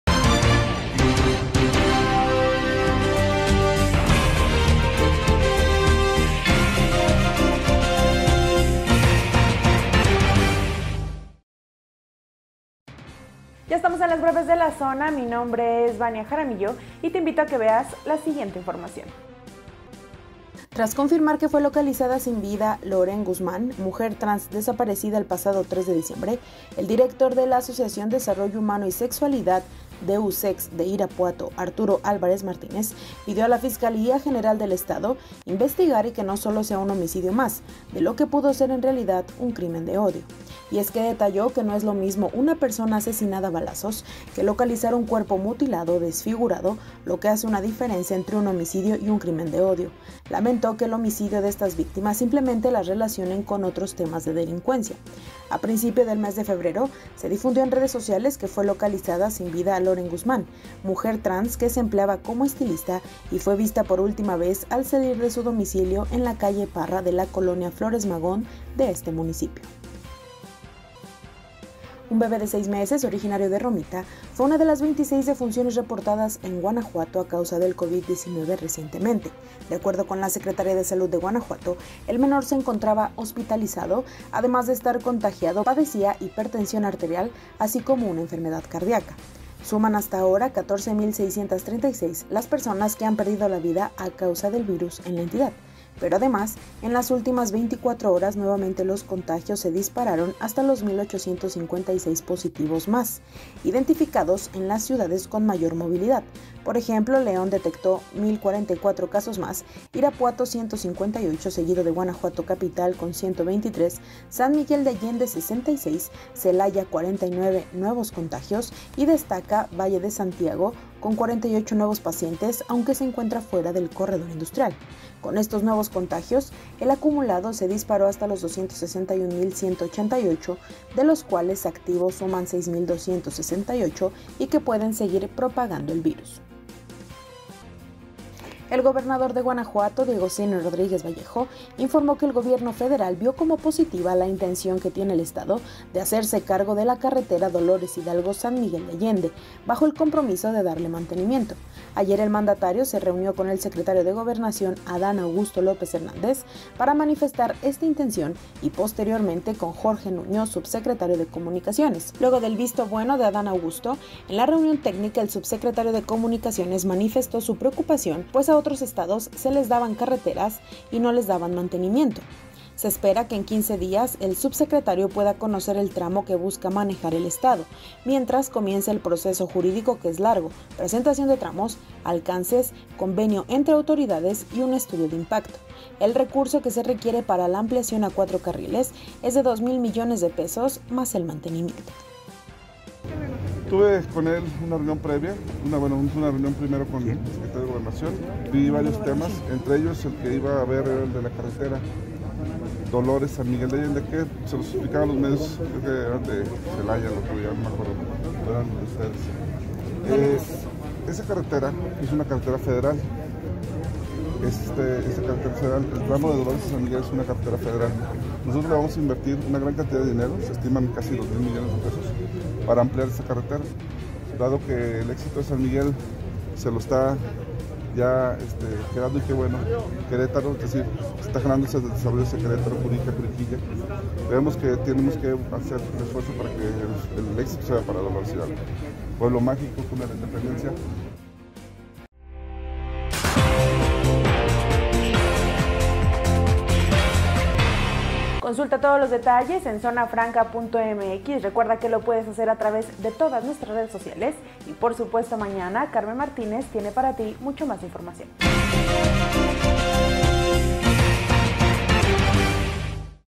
Muere otro bebé por COVID-19 en Guanajuato | BrevesDeLaZona📰
Esto es LoMásDestacado de este jueves
- Hallan sin vida a mujer trans; sospechan sea un crimen de odio
- Federación apoya al Estado para que se haga cargo de carretera Dolores Hidalgo - SMA